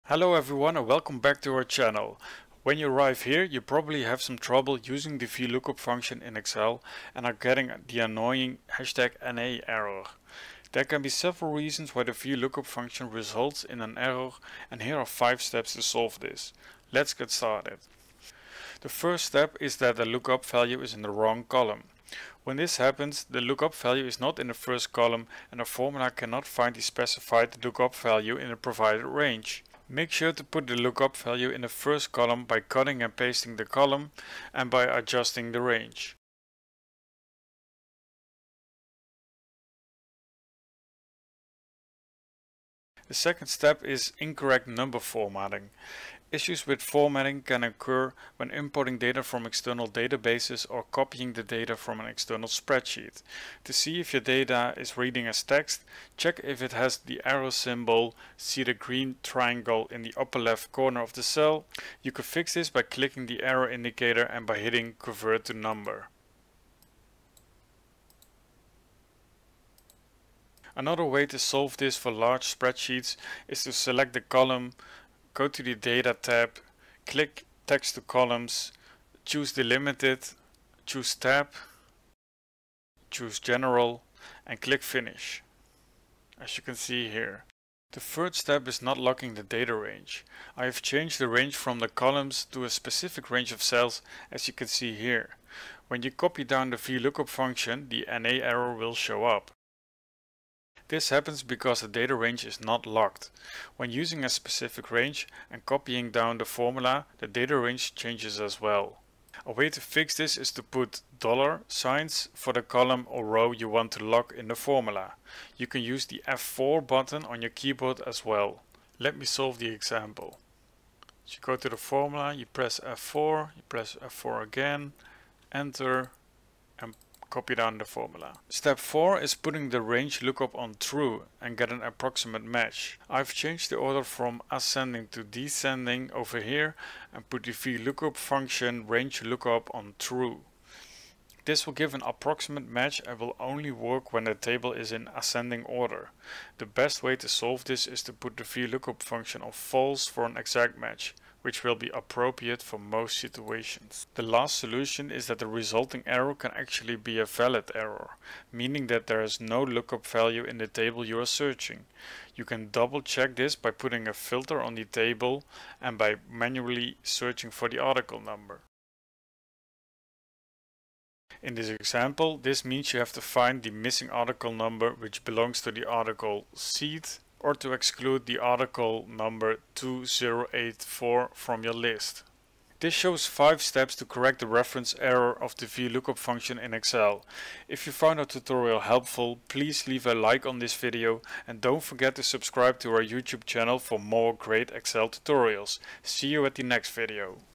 Fix The VLOOKUP Error N/A in Excel
Learn how to quickly fix the N/A error of VLOOKUP in Excel with this step-by-step tutorial!

In this Excel tutorial, you will learn how to fix the # N/A error of the VLOOKUP formula with 5 tips. When VLOOKUP is simply not working, gives the n/a error and you didn't know how to fix it: this video is for you! We will explore different tips to overcome this common VLOOKUP error and ensure accurate data retrieval in Excel. Don't let the na error hold you back, watch now and become an Excel pro!

You might wonder: Why do we get an error in Vlookup in Excel? This tutorial is for you when you have Vlookup n/a error problems and Vlookup is not working. With these 5 solutions you can fix the ref error and keep enjoying VLOOKUP in Excel.

Timestamps
00:00 VLOOKUP Problems and how to Fix them!
00:18 Lookup Value in the wrong column
00:43 Incorrect number formatting error
01:11 Fix for Large Datasets - Incorrect number formatting
01:28 Not locking the data range
02:08 Range lookup is on TRUE
02:34 It might be a valid error?!
03:02 Further assistance